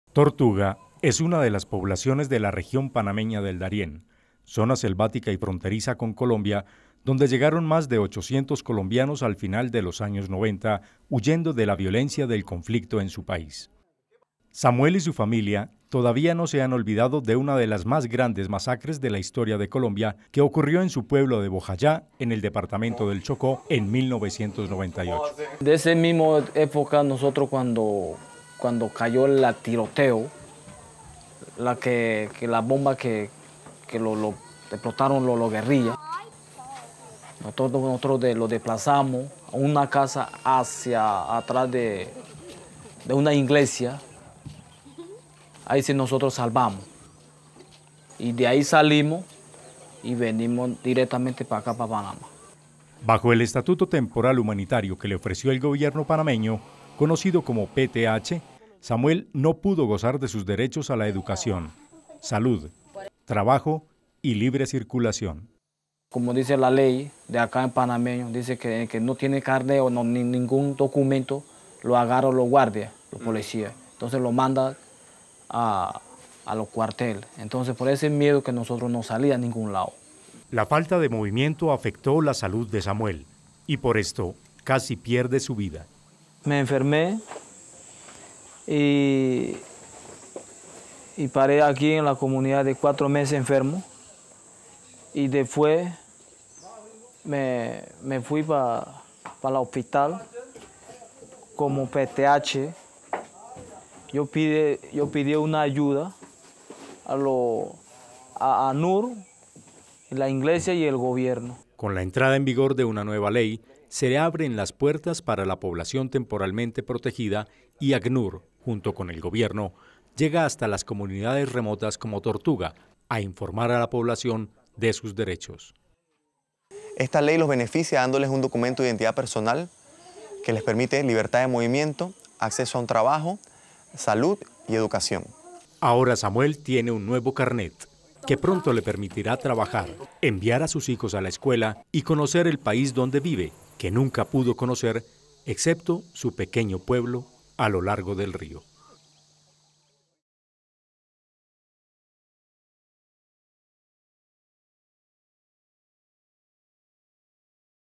Panamá: Una nueva vida para los refugiados
Jaqué, El Real, Puerto Obaldía, Boca de Cupe, Yape, Yaviza y la Palma son algunas de las poblaciones que albergan alrededor de 863 personas que ingresaron a finales de 1996 al Darién panameño huyendo de la violencia relacionada con el conflicto en Colombia. Para su protección en Panamá, fue concedido por el Estado un estatuto particular llamado el "Estatuto Humanitario Provisional de Protección" que les proporcionaba el derecho a vivir en el país de manera temporal. Para la población refugiada esta situación es doblemente crítica, pues el estatuto temporal no incluye el derecho al trabajo, salud y educación. El largo camino hacia una solución duradera se concretó en  diciembre de 2011 con la entrada en vigor de la ley que establece el cambio de la categoría migratoria para la población temporalmente protegida, abriendo las puertas para que opten por su residencia permanente y, finalmente, su integración plena en Panamá. Aún con este estatuto más favorable, las 863 personas que se benefician directamente con esta ley continúan siendo personas con necesidades de protección.